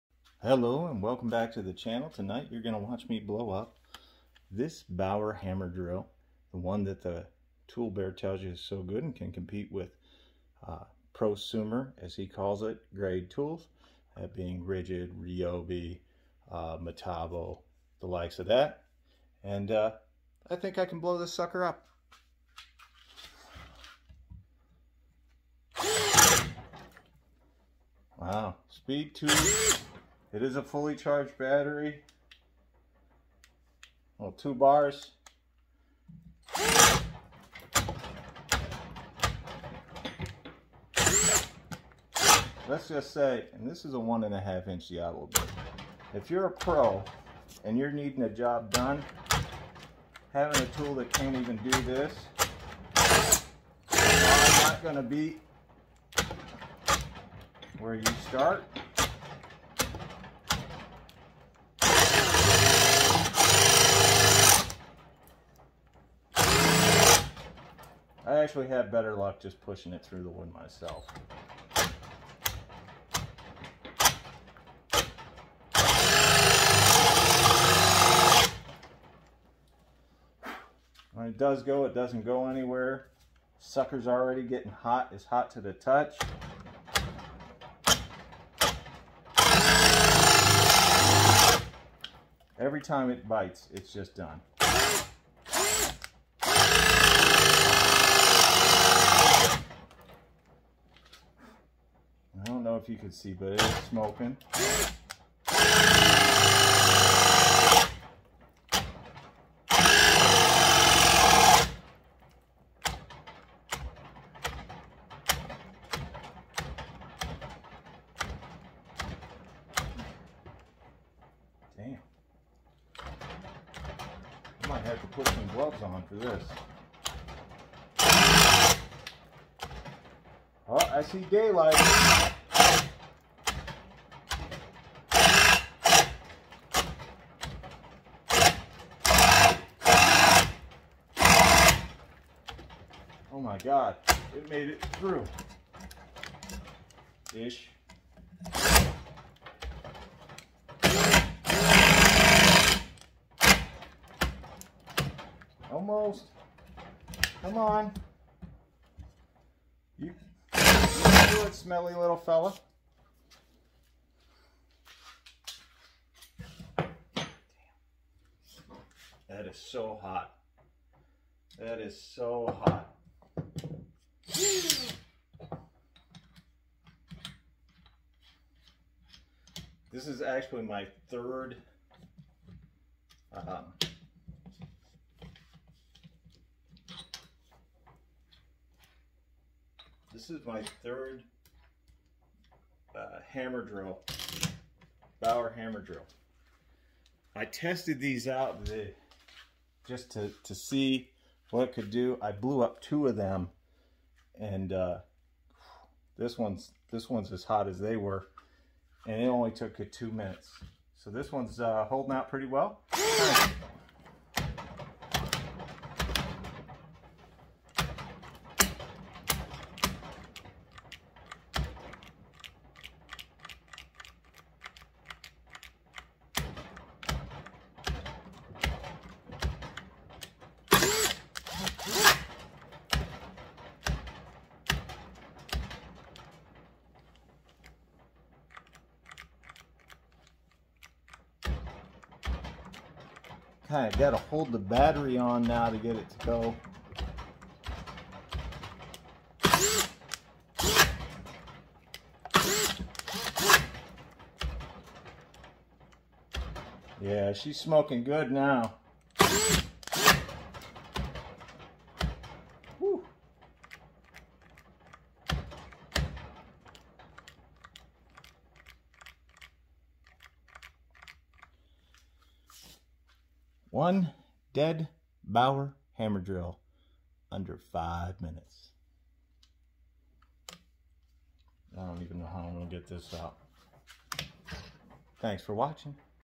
Bauer Hammer Drill goes up in smoke!
Exposing the most underpowered hammer drill on the market and proving it is not a prosumer grade tool in two easy steps. I did end up stepping it down to speed one and the result was the same. This thing is a POS!